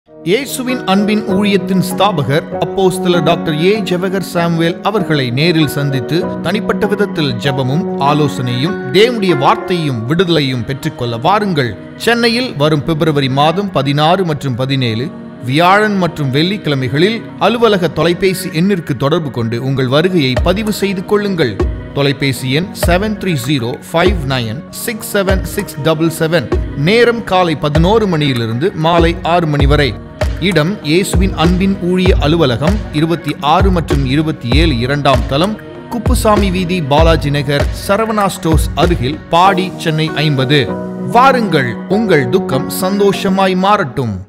Chennai Appointment Prayer | 16th & 17th February 2023 | Apostle Dr A Jawahar Samuel
Love of Jesus Ministry Presents

Chennai Appointment Prayer | 16th & 17th February 2023

Venue : Love of Jesus Ministry ( Chennai )